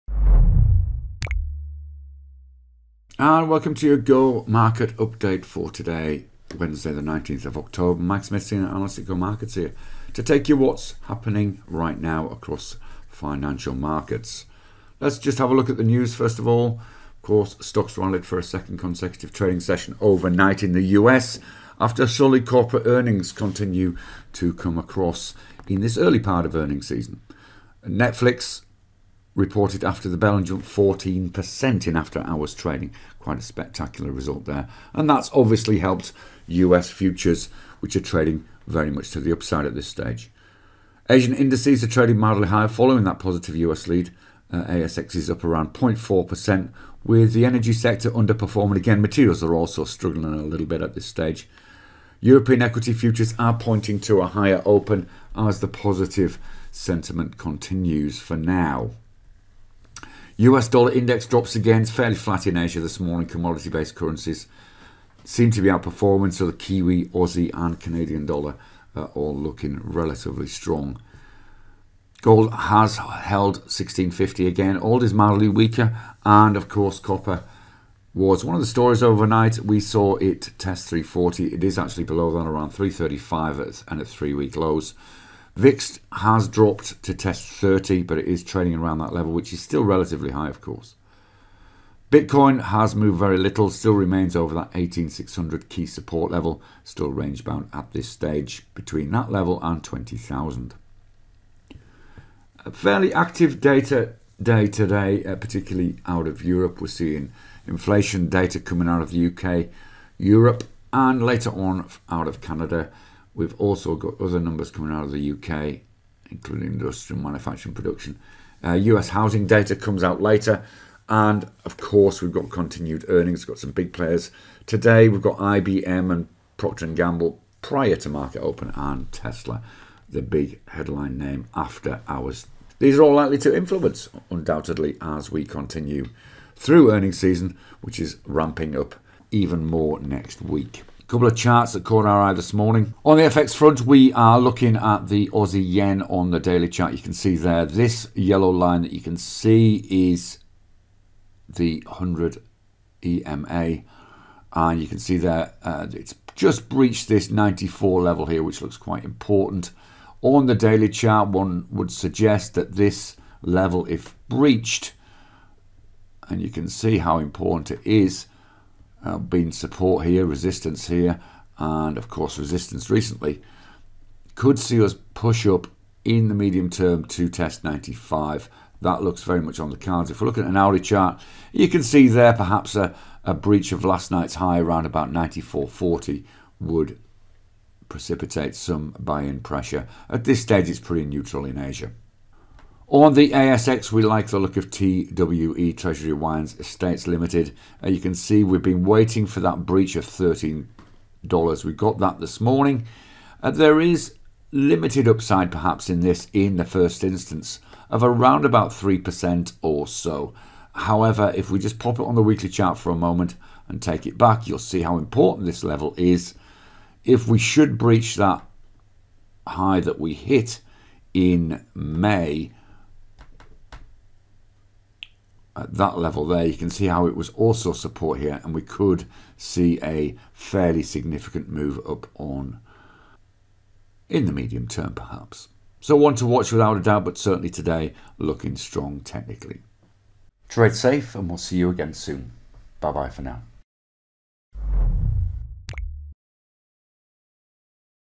GO Market Update and charts of interest - AUDJPY and TWE
Stocks rallied for a second consecutive trading session overnight after solid corporate earnings. NFLX jumps 14% in after hours trading.